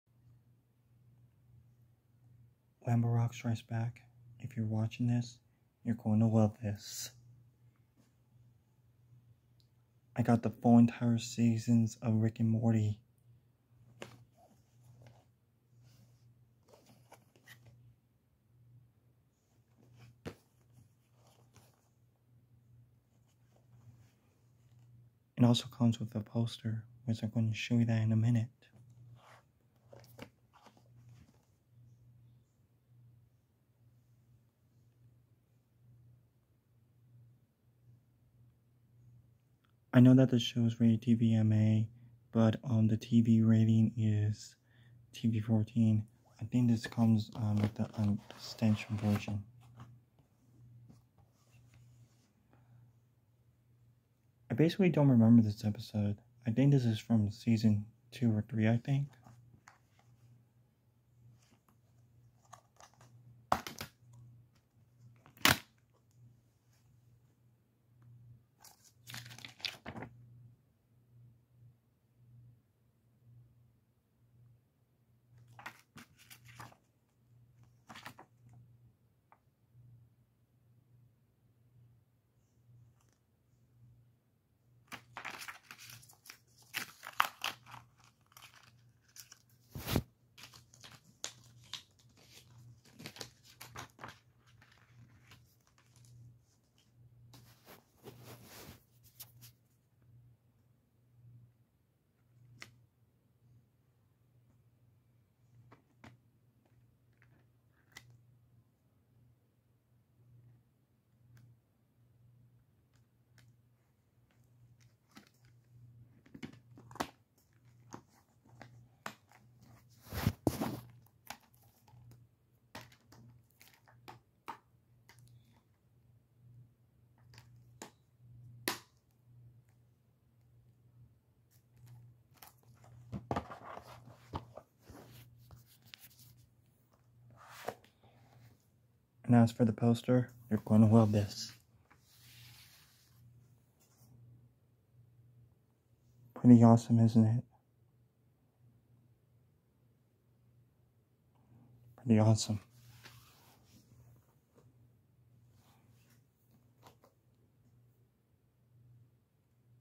my DVD update for July 18 2021
I got rick and morty on DVD finally 🤩🤩🤩